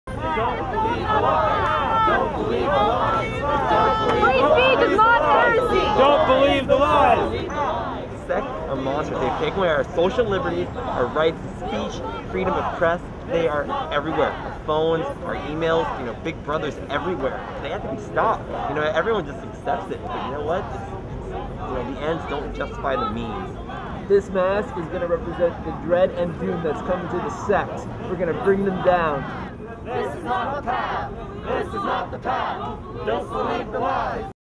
Anti-Sect Protest
A video of one of the many Sect protests occurring across the globe.